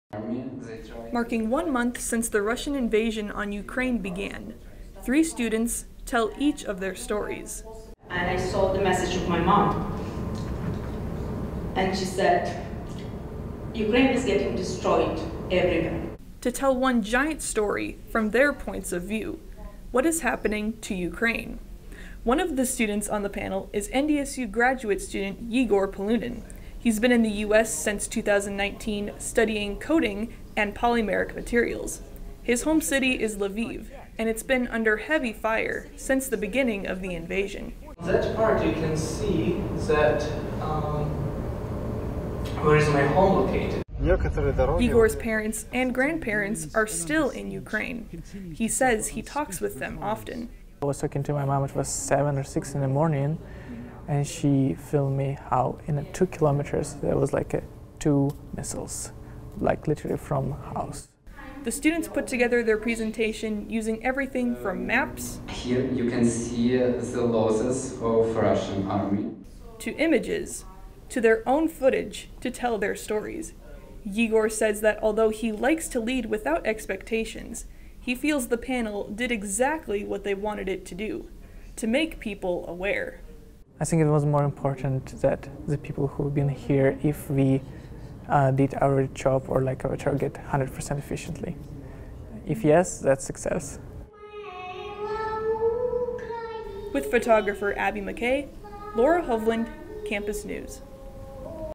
NDSU Ukraine Panel
NDSU hosts a panel of Ukrainian students sharing their thoughts on the ongoing conflict in Ukraine.
Reporter: Laura Hovland
Photographer: Abby Makay